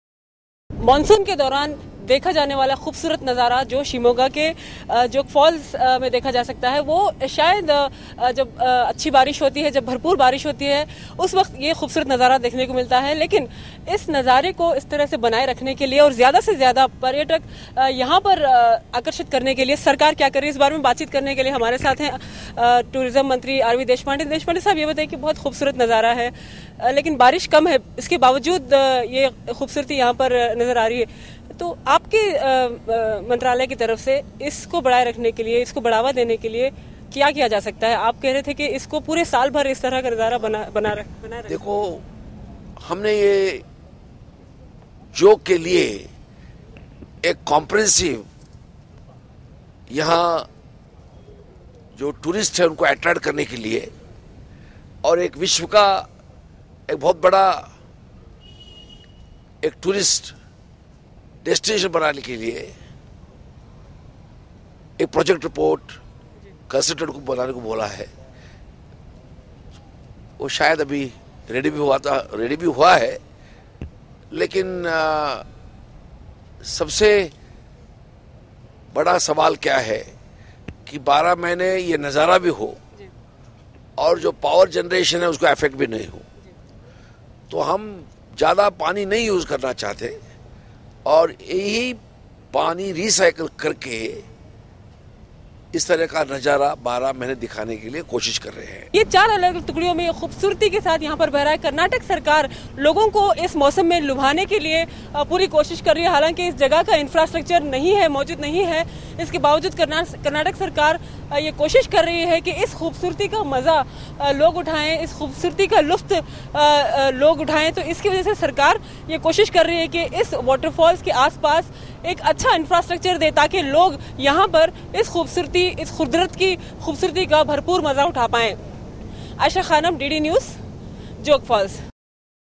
Karnataka: Interview with state Tourism Minister R.V. Deshpande
Exclusive: Interview with Karnataka state Tourism Minister R.V. Deshpande.